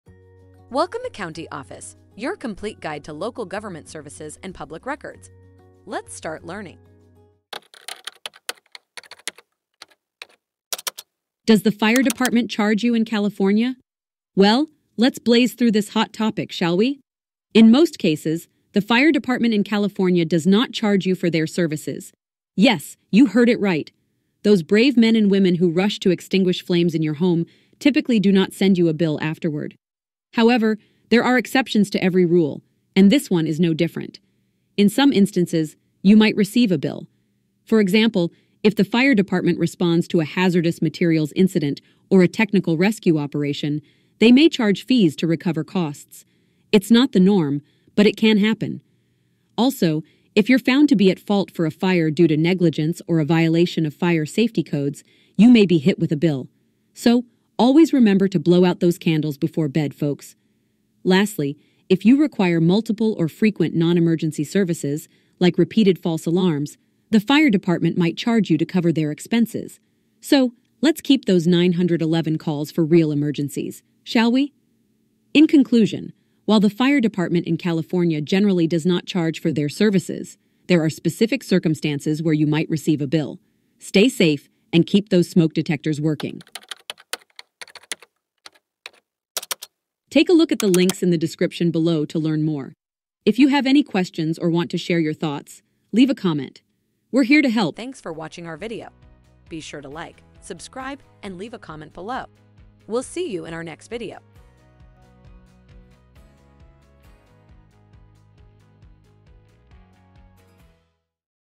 Does The Fire Department Charge You In California? - CountyOffice.org
Does The Fire Department Charge You In California? In this enlightening video, we'll demystify the burning question of whether the fire department charges for their services in California. We dive into the circumstances under which you might receive a bill and how to avoid it.

We'll start by revealing that, in general, the fire department does not charge for their services. You'll learn about the heroic men and women who risk their lives to extinguish fires without expecting a bill in return.

However, there are exceptions to this rule. We'll delve into scenarios such as hazardous materials incidents, technical rescue operations, and violations of fire safety codes, where you might be billed. Additionally, we'll discuss how repeated false alarms or non-emergency services could result in a charge.

Whether you're a homeowner, a tenant, or simply a curious bystander, understanding these nuances is crucial.